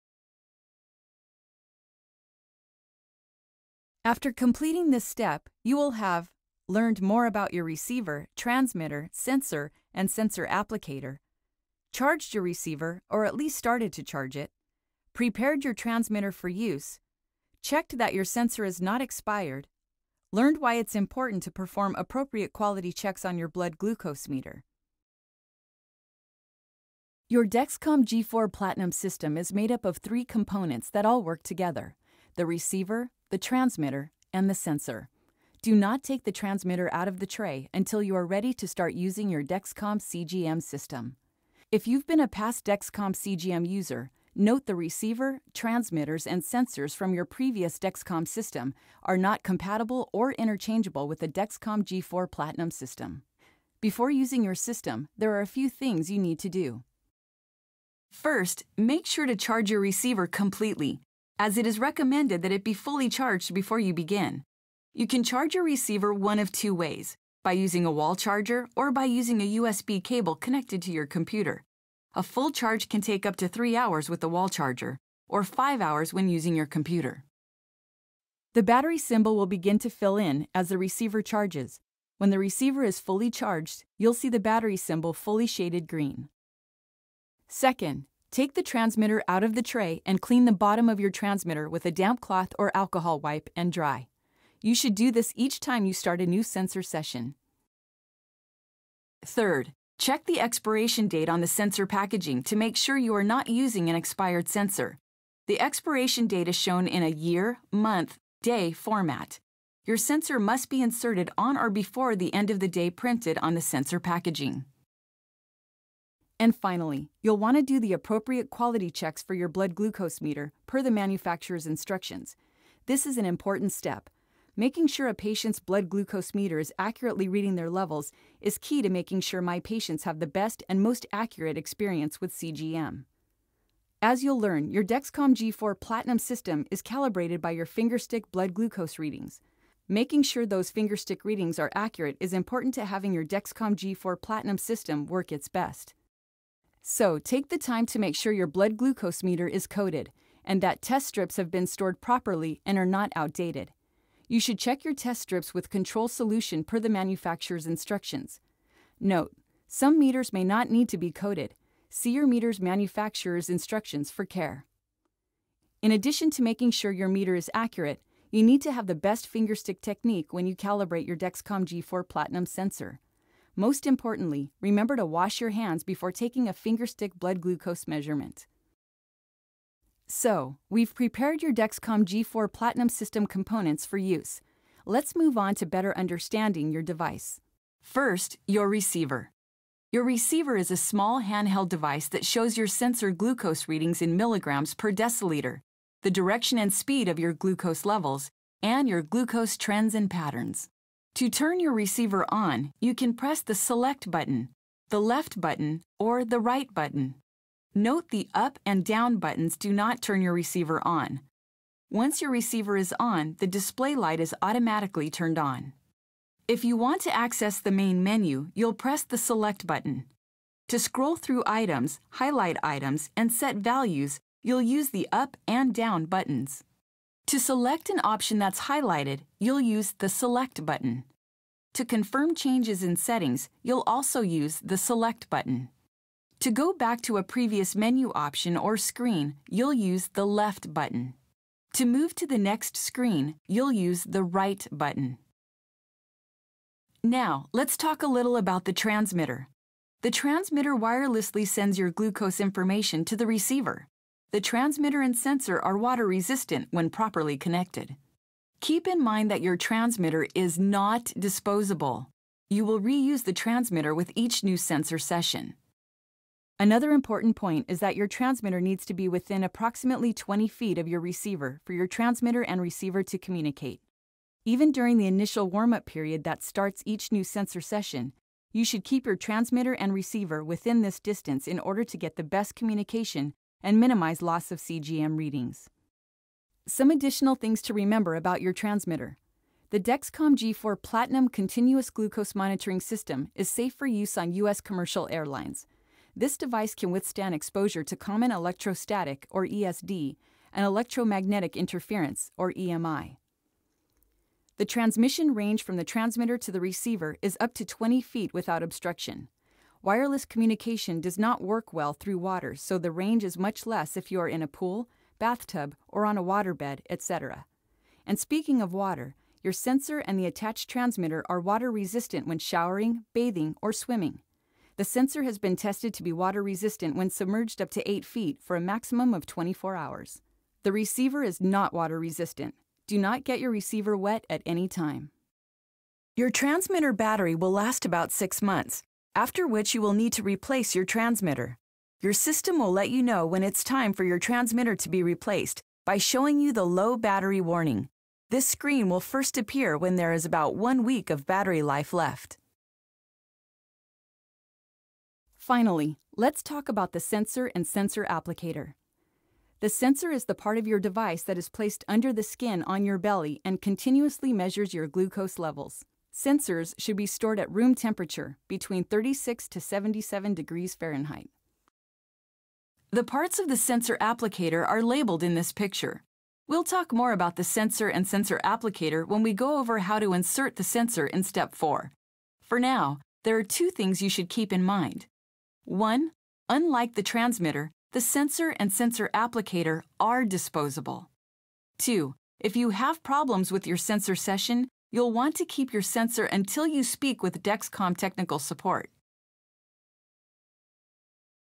Dexcom G4 Platinum Continuous Glucose Monitoring System With Share
Dexcom G4 Platinum Continuous Glucose Monitoring System With Share is equipped with the bluetooth technology which is built into the receiver for remote glucose monitoring. This wireless technology allows easy viewing of glucose levels between the person with diabetes and their loved ones from an Apple iPhone or iPod touch. The trend arrows and rate-of-change alerts catches high and lows before they happen while with the touch of a button the user can know where glucose is, where it’s going, and how fast it’s getting there. It is discreet, comfortable, and lifestyle friendly.